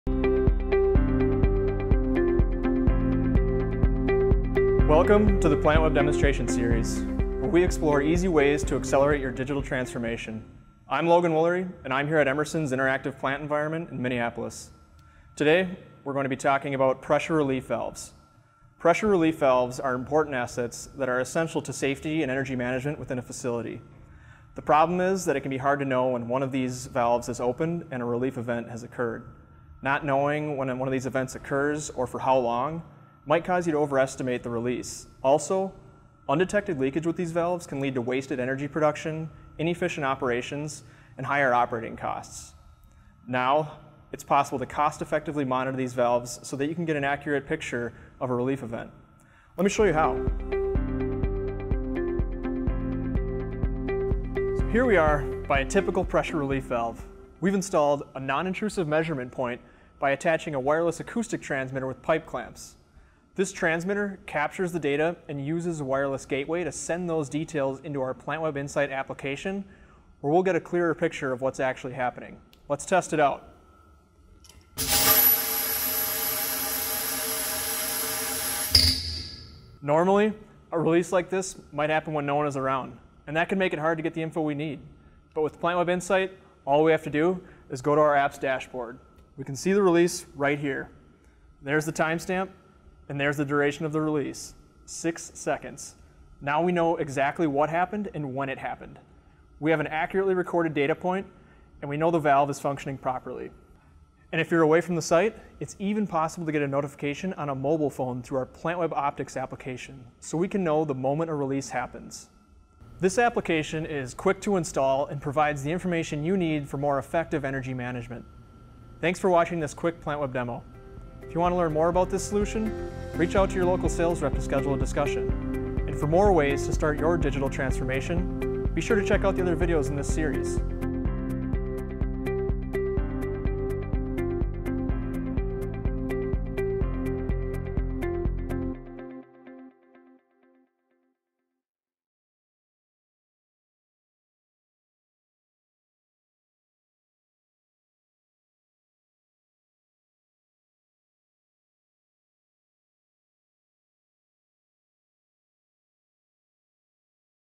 Enhancing Visibility into Pressure Relief Valve Events
Discover how to prevent wasted energy and undetected leakages from your pressure relief valves by applying wireless condition monitoring that helps you know the exact moment a release occurs and its duration so that you can meet performance goals.